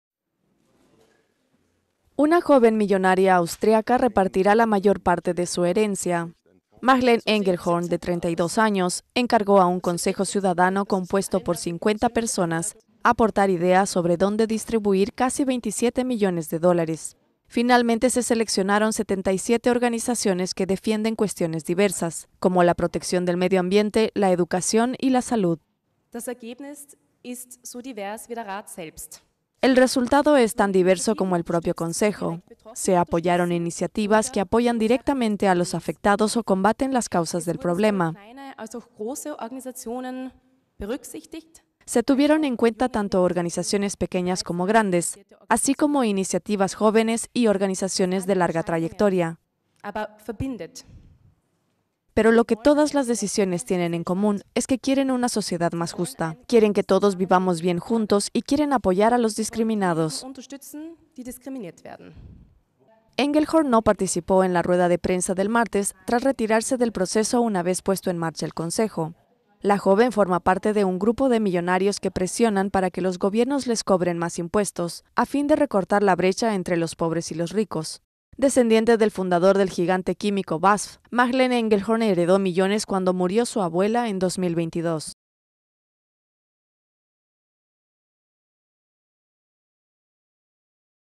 Una joven millonaria repartirá su fortuna entre 77 organizaciones
La millonaria austriaca Marlene Engelhorn, de 32 años, repartirá casi 27 millones de dólares, la mayor parte de su fortuna, entre 77 organizaciones seleccionadas por un panel de 50 ciudadanos.